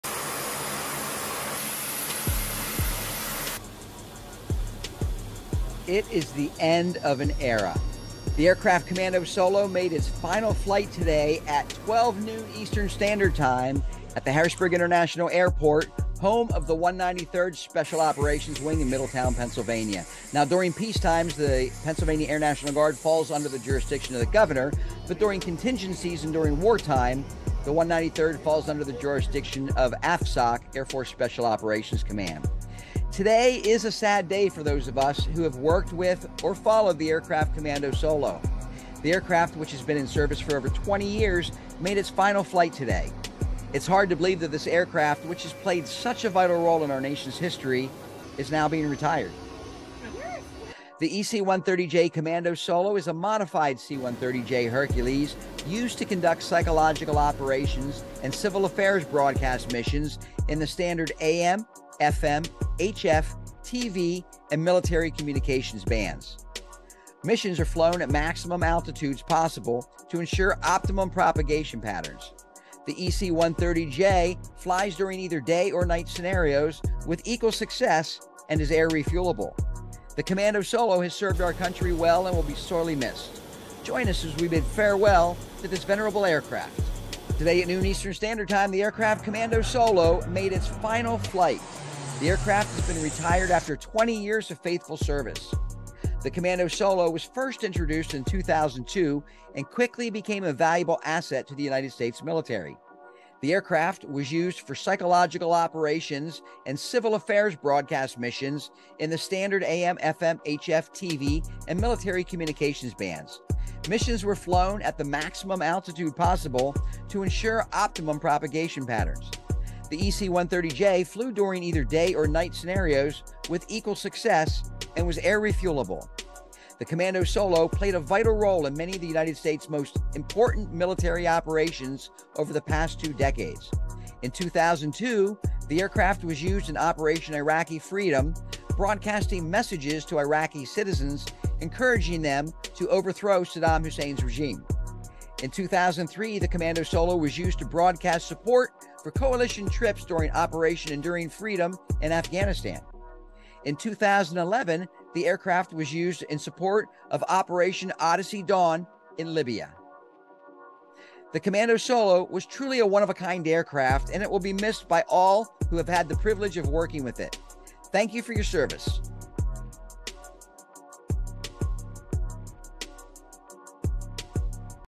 The End of an Era: Aircraft Commando Solo Makes Its Final Flight Today
Some public domain video portions recorded by MSGT Matt Schwartz, 193d SOW

 Introduction: Today is a sad day for those of us who have worked with or followed the Aircraft Commando Solo. The aircraft, which has been in service for over 20 years, made its final flight today. It's hard to believe that this aircraft, which has played such a vital role in our nation's history, is now being retired.

The EC-130J Commando Solo is a modified C-130J Hercules used to conduct psychological operations (PSYOP) and civil affairs broadcast missions in the standard AM, FM, HF, TV and military communications bands. Missions are flown at the maximum altitudes possible to ensure optimum propagation patterns. The EC-130J flies during either day or night scenarios with equal success, and is air-refuelable.

The Commando Solo has served our country well, and will be sorely missed. Join us as we bid farewell to this venerable aircraft.

Today at noon EST, the Aircraft Commando Solo MADE its final flight. The aircraft has been retired after 20 years of faithful service.

The Commando Solo was first introduced in 2002, and quickly became a valuable asset to the United States military. The aircraft was used for psychological operations (PSYOP) and civil affairs broadcast missions in the standard AM, FM, HF, TV and military communications bands. Missions were flown at the maximum altitudes possible to ensure optimum propagation patterns. The EC-130J flew during either day or night scenarios with equal success, and was air-refuelable.

The Commando Solo played a vital role in many of the United States' most important military operations over the past two decades. In 2002, the aircraft was used in Operation Iraqi Freedom, broadcasting messages to Iraqi citizens encouraging them to overthrow Saddam Hussein's regime. In 2003, the Commando Solo was used to broadcast   support for coalition troops during Operation Enduring Freedom in Afghanistan. In 2011, the aircraft was used in support of Operation Odyssey Dawn in Libya.

The Commando Solo was truly a one-of-a-kind aircraft, and it will be missed by all who had the privilege of working with it. Thank you for your service!

Thank you for joining us as we bid farewell to the Aircraft Commando Solo! This
 aircraft has served our country well for over 20 years, and will be sorely missed by all who had the privilege of working with it.